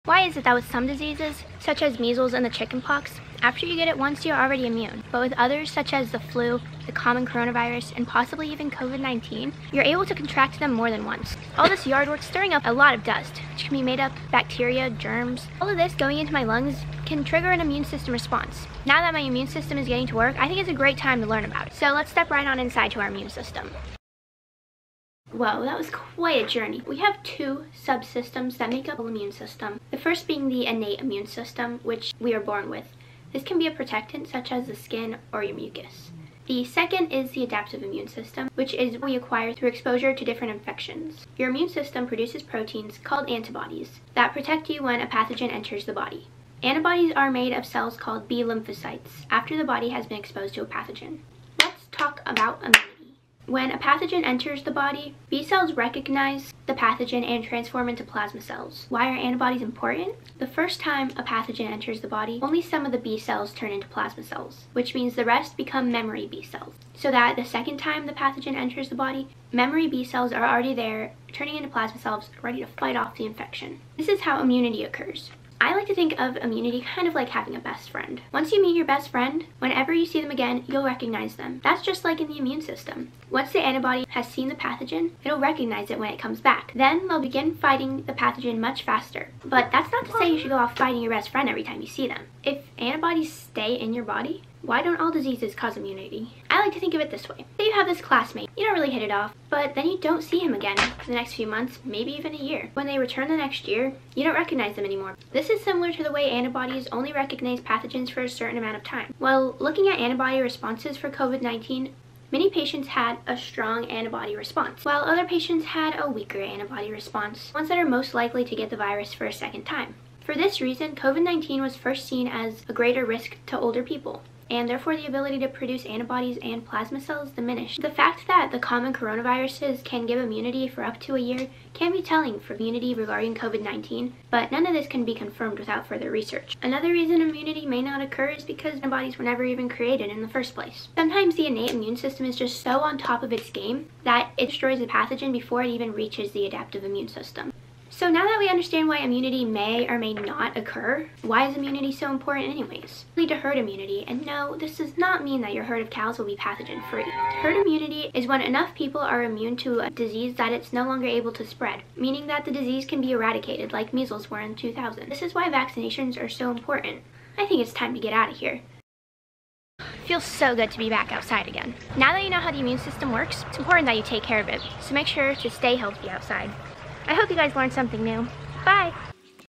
IMMUNOLOGY: disease immunity and COVID-19
in this video, I explain the adaptive immune system and how you can become immune to certain diseases, and why immunity may or may not occur